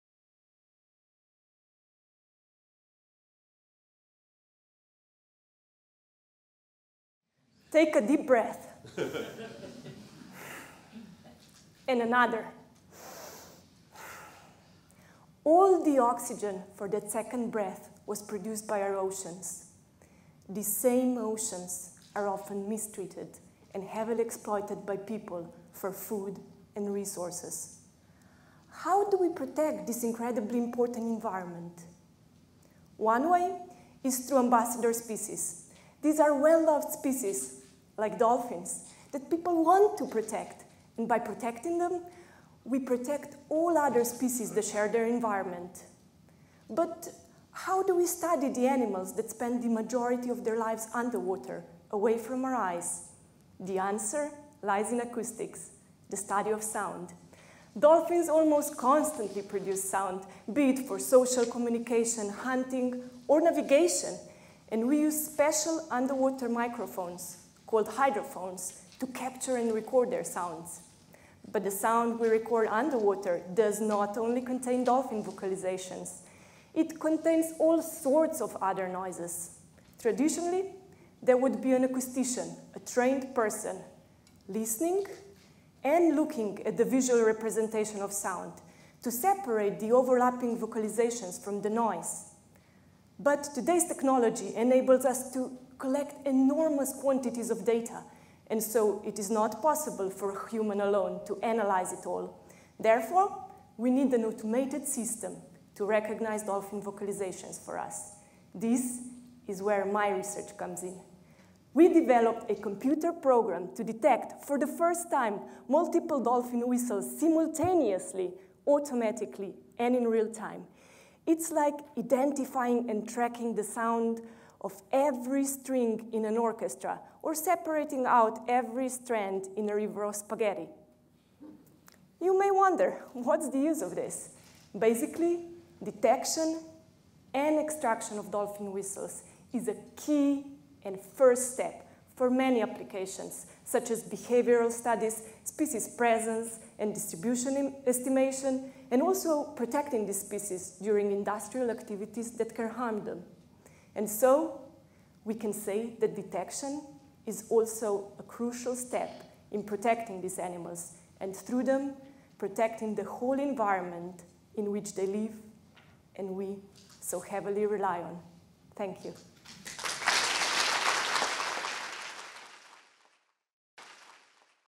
Three Minute Thesis '16, Pina Gruden | University of Southampton
Pina Gruden from the Faculty of Engineering and Environment discussed her research on ' the automated detection of dolphin whistles' in three minutes. In May 2016, eight doctoral researchers competed in the University’s Three Minute Thesis competition.

First developed by the University of Queensland and now an international initiative, this competition requires doctoral researchers to explain their research project to a non-specialist audience in just three minutes.